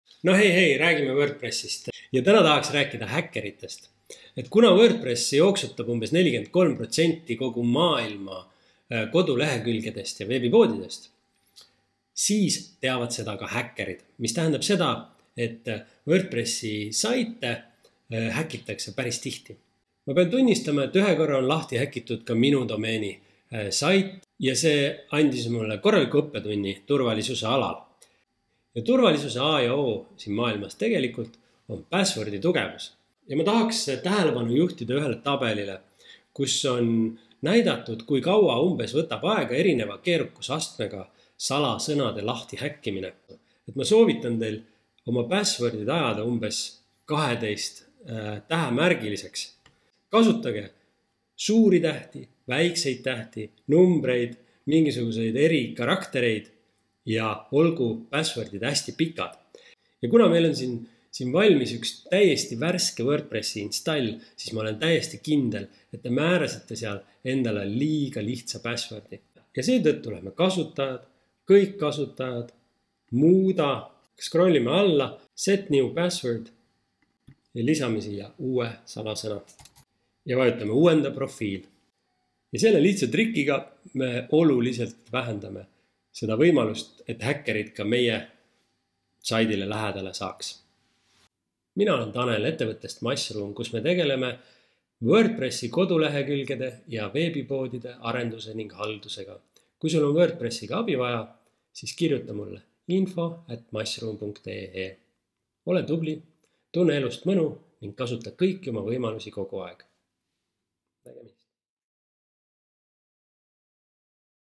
Mega oluline WordPressi turvanipp - kuidas tõsta oluliselt kodulehe turvalisust?
Osa 2. Mega olulise turvanipi toob teieni Tanel Teemusk.
See on tegelikult suht universaalne turvanipp, mitte ainult Wordpressi jaoks. Kasuta vastutustundeliselt!

- Kui registreerid domeeni, siis tee seda ja kasuta promokoodi “MASSRUUM“. Saad räigelt hea hinna (ja ma saan ka vist midagi).